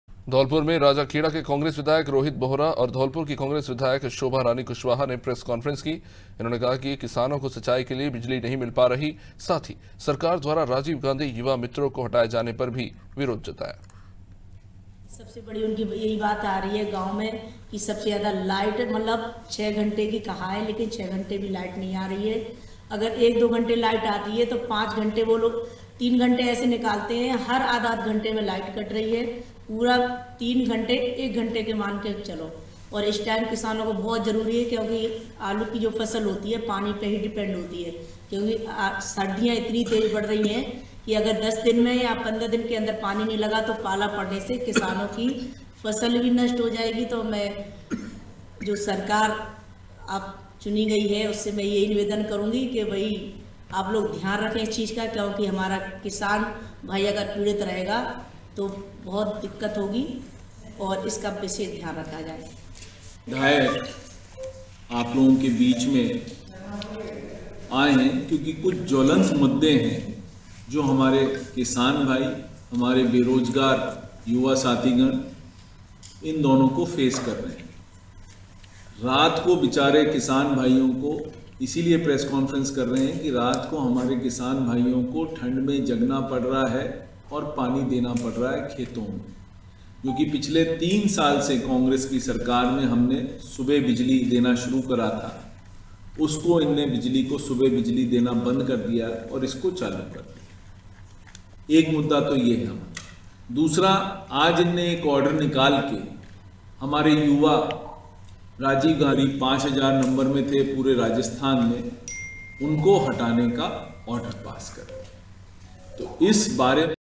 Dholpur : कांग्रेस ने की प्रेस कॉन्फ्रेंस, बीजेपी पर साधा निशाना | Hindi News || Anaadi Tv
धौलपुर में राजाखेड़ा के कांग्रेस विधायक रोहित बौहरा और धौलपुर की कांग्रेस विधायक शोभारानी कुशवाहा ने प्रेस कॉन्फ्रेंस की है...कांग्रेस का कहना है कि किसानों को सिंचाई के लिए बिजली नहीं मिल पा रही है...साथ ही सरकार द्वारा राजीव गांधी युवा मित्रों को हटाए जाने पर भी विरोध जताया है..

AIRTEL - 379